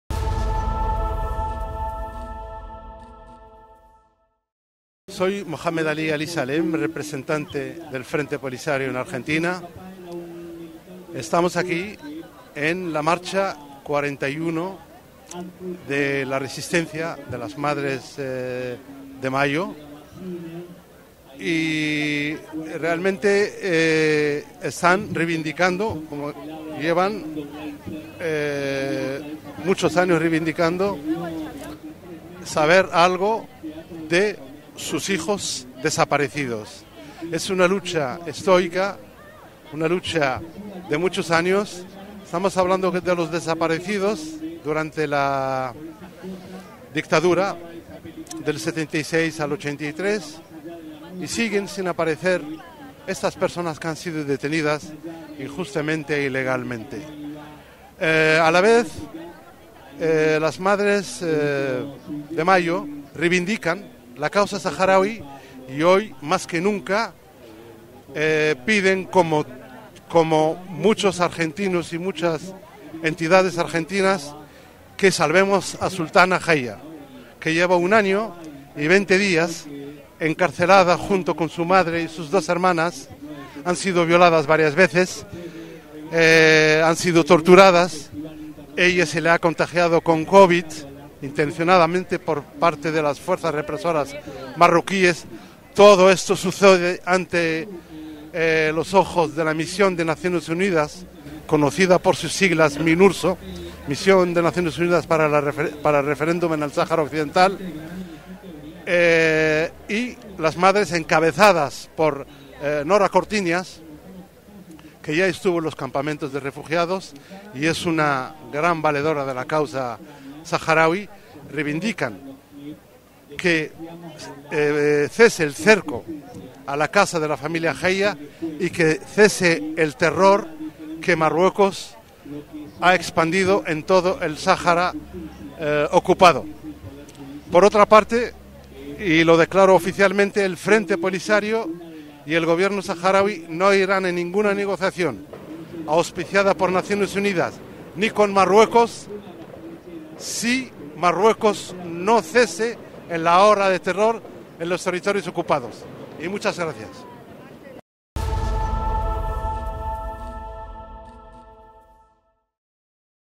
Argentina. Piden por Sultana Khaya en Marcha de la Resistencia
Las Madres de Plaza de Mayo se suman a la campaña por la libertad de Sultana Khaya, activista saharaui torturada en los territorios ocupados por Marruecos.